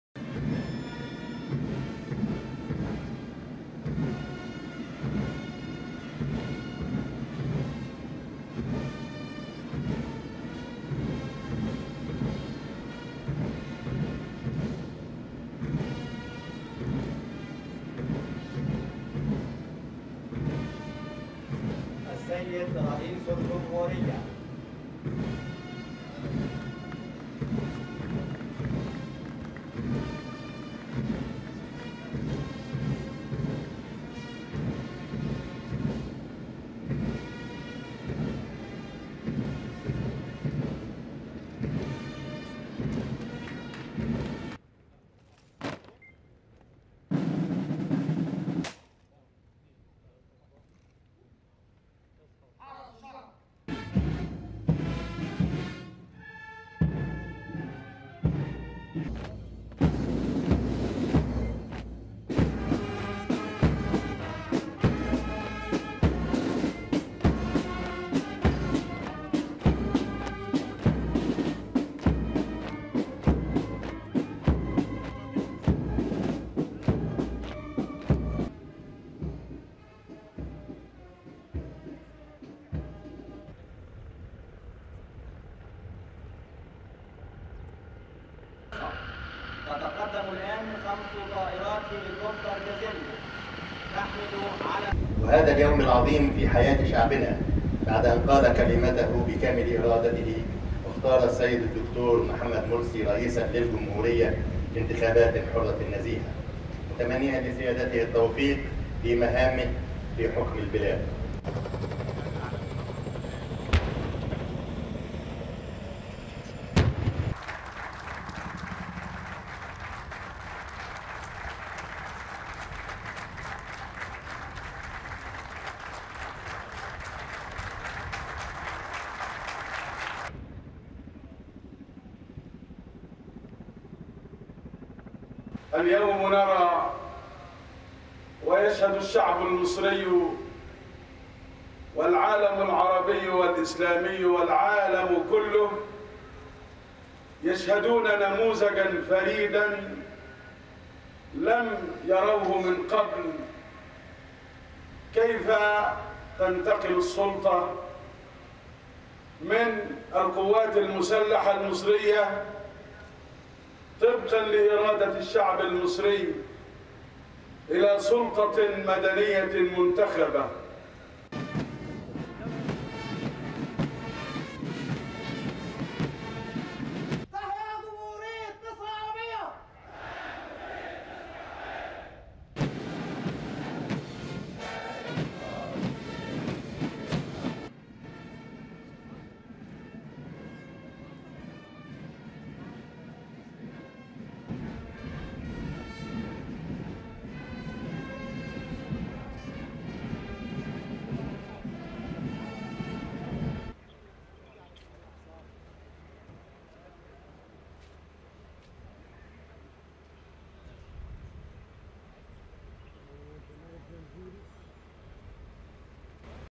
New Egypt President Morsi attends military parade
(30 Jun 2012) A few hours after Egypt's new president Mohammed Morsi was sworn in on Saturday, the ruling military council formally handed over power to the country's first freely elected leader.
The military council under Field Marshall Mohamed Hussein Tantawi had ruled the most populous Arab state since Hosni Mubarak's overthrow in February 2011.
Tantawi, who saluted Morsi when he arrived at the Heikstep army base just outside Cairo for the televised occasion, said the military council had kept their promise to hand over power.
"On this great day in the lives of our people, after having their say with complete will, and choosing Dr. Mohamed Morsi as a president of the republic in free and fair elections, we wish him success in his mission of ruling the country," he said.
The two men stood together on the dais, surrounded by army officers, aides and security men, as they watched a colourful ceremonial marchpast, with only a gun salute of tank fire to remind anyone of the army's real power.
Despite the parade and the speeches, Egypt remains in political limbo, without a constitution, a lower house of parliament or any clarity about the role of a military.
Commentators say the army shows every sign of reluctance to withdraw from politics but Islamists and democracy campaigners are insisting that it does.
Tantawi's Supreme Council of the Armed Forces (SCAF) has guided the country since Mubarak's overthrow, holding elections, but issuing arbitrary and often contradictory decrees, while the economy shrank, increasing hardship for millions of Egyptians.
An army decree on 17 June, 2012, cut presidential powers, denying the head of state his role as supreme commander of the armed forces with the right to decide on war and peace.
It also gave SCAF legislative powers until a new parliament is elected and veto rights over the terms of a new constitution.
Morsi, a bearded engineer who turned to the Muslim Brotherhood as a graduate student in Los Angeles, California, is Egypt's first civilian leader since army officers toppled King Faroukh in 1952.
"Today, the Egyptian people, the Arab and Islamic world, and the whole world, witnesses a never before seen, a unique model of how power is transferred from the Egyptian armed forces - by the will of the Egyptian people - to an elected, civilian power," Morsi said.
For the 84-year-old Brotherhood, banned and repressed by Mubarak, his inauguration marks a dramatic reversal of fortunes.